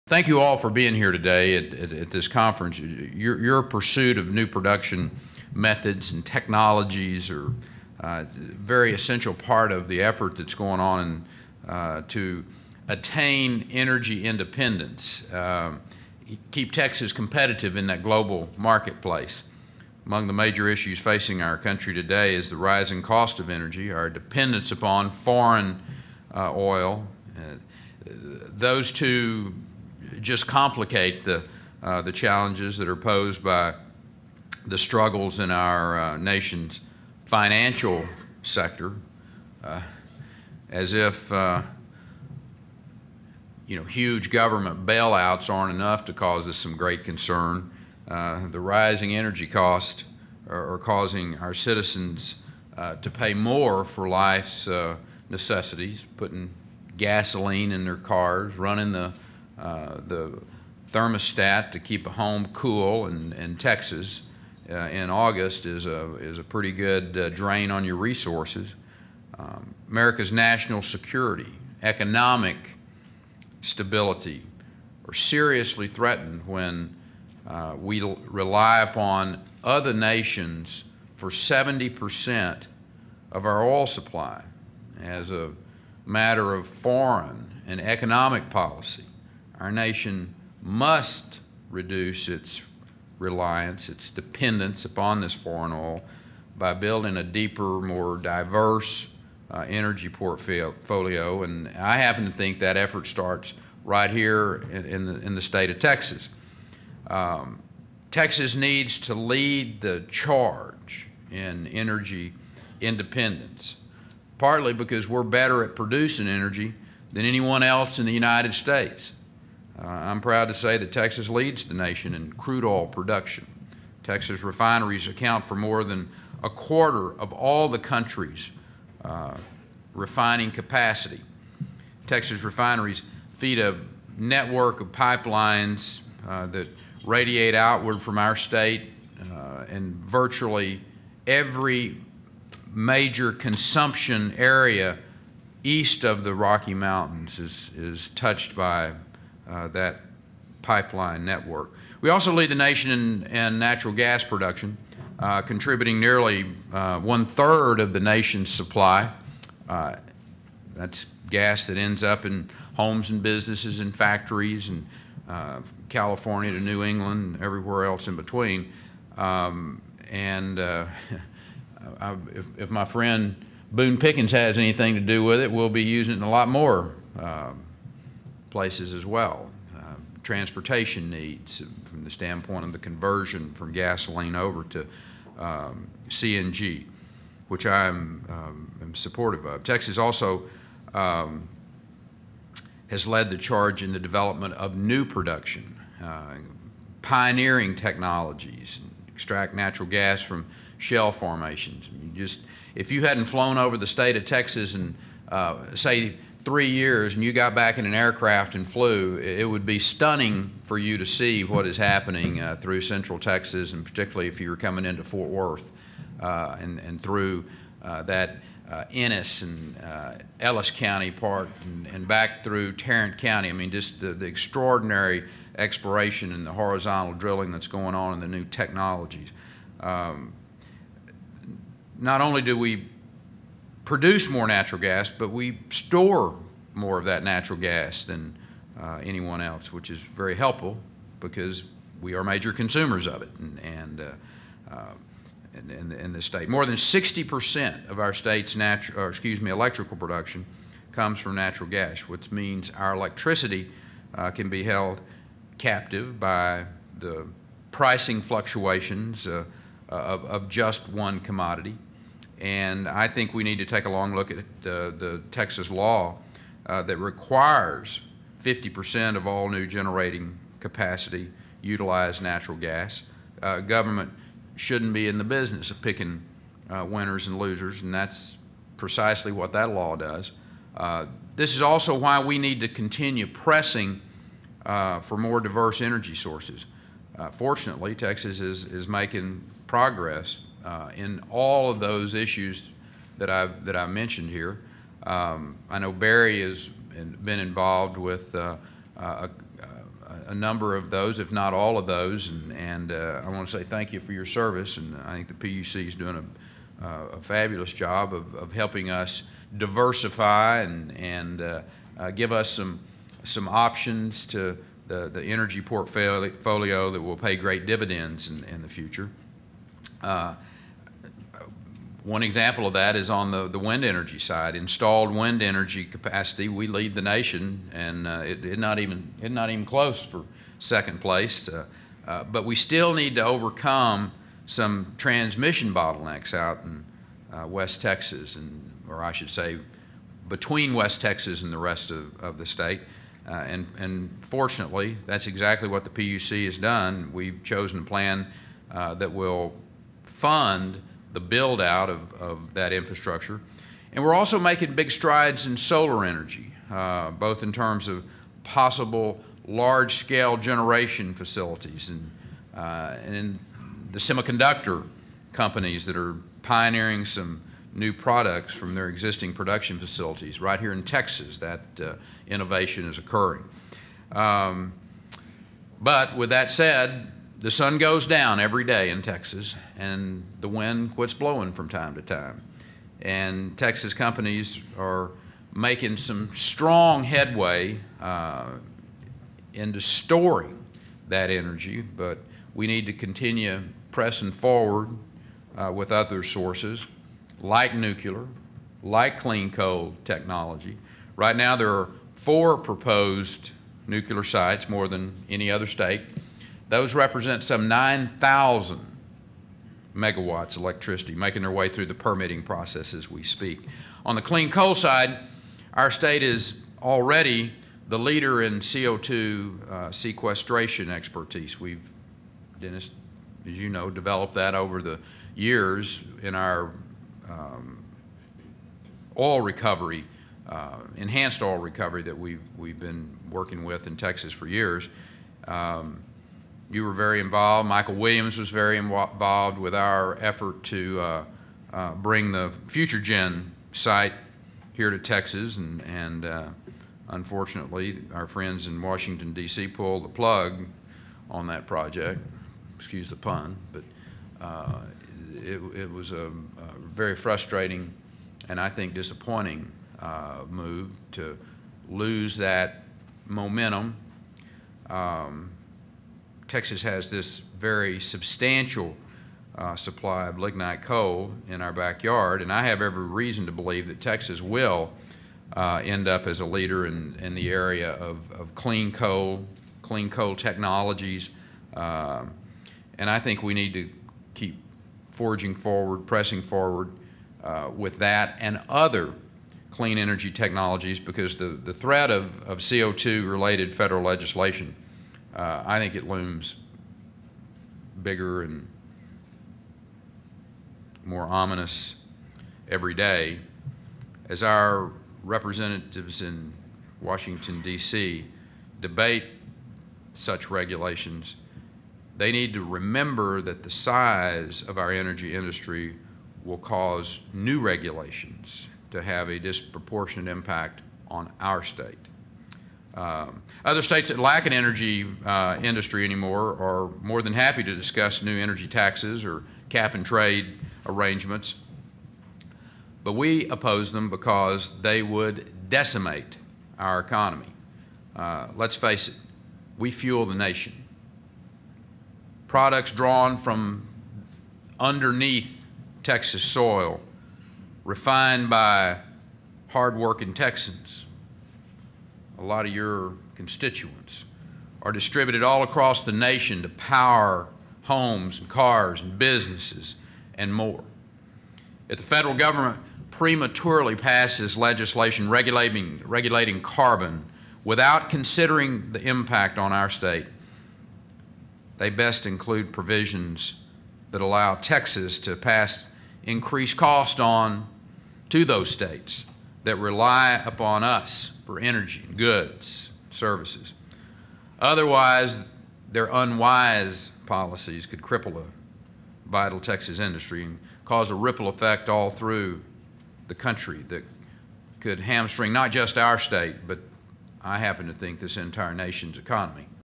Gov. Perry: Texas Needs To Lead The Charge On Energy Independence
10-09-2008 - AUSTIN - Gov. Rick Perry today told energy experts that as a matter of foreign and economic policy, the nation must reduce its dependence on foreign oil by building a deeper, more diverse energy portfolio.

Key Points:
Gov. Perry says Texas must lead the charge in energy independence

Governor spoke to Clean Carbon Policy Summit and Project Expo

Governor emphasized energy diversification to reduce dependence on foreign supplies